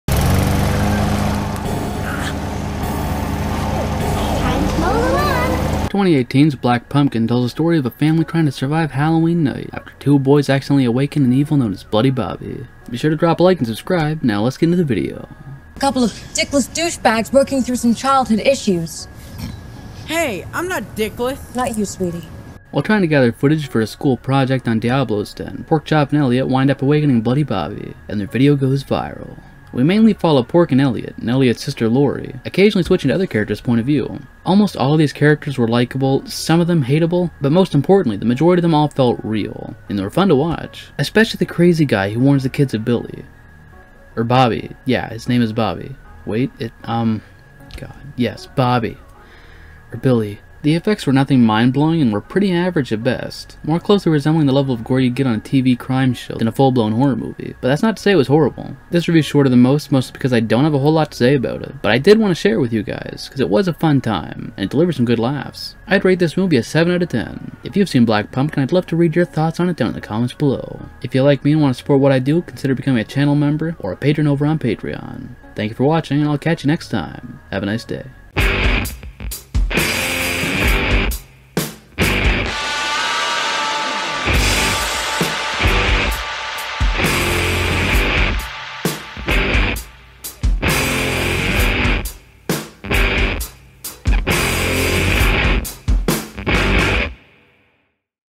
Black Pumpkin (2018) Movie Review - A Great Halloween Movie!
I wanted to share this fun Halloween movie with you guys! Today I'm doing a quick review of Black Pumpkin (2018)